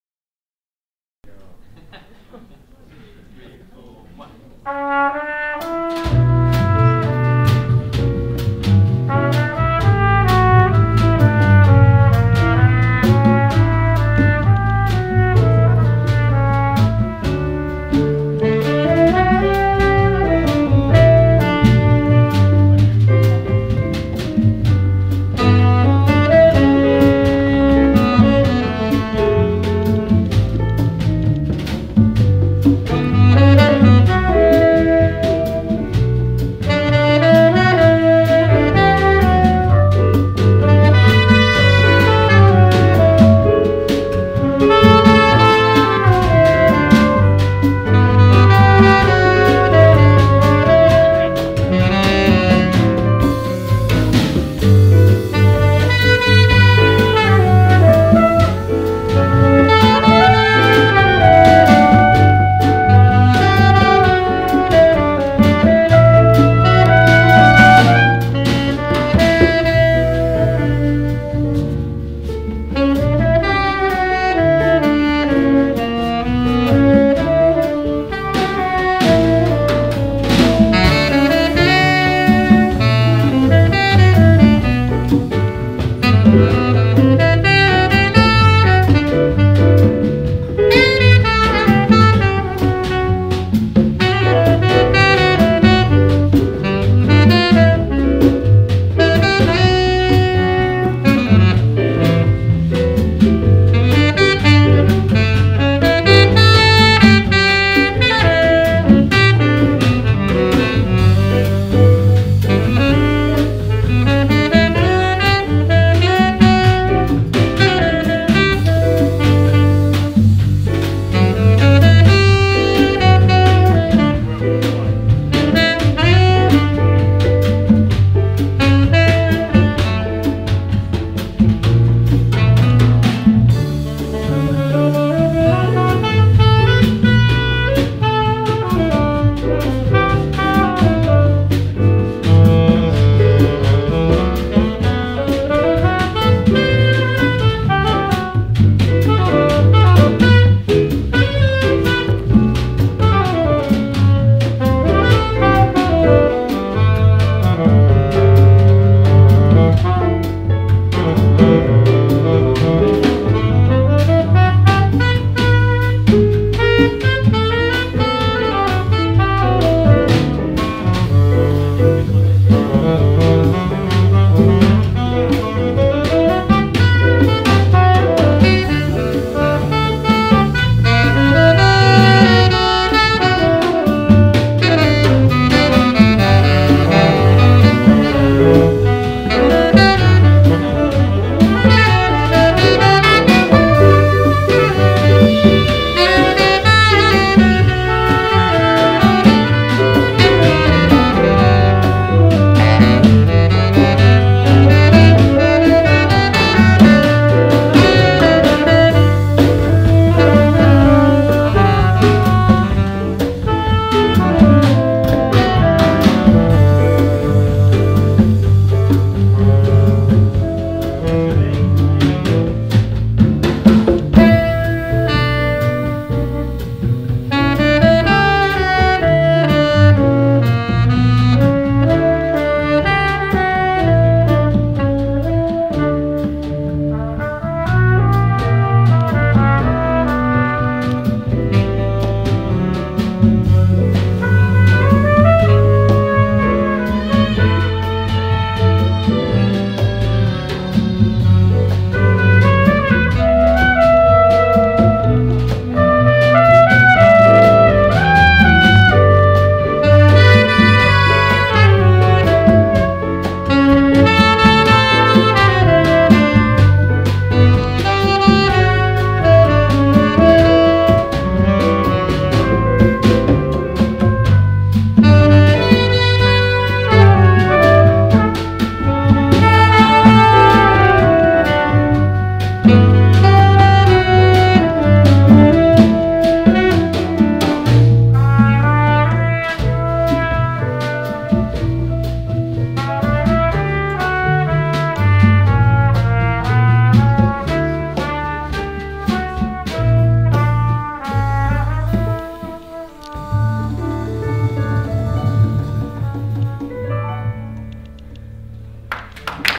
Julian Dean South African Jazz Quintet with guests
At Theatre Severn - Lunchtime Jazz 24/11/12.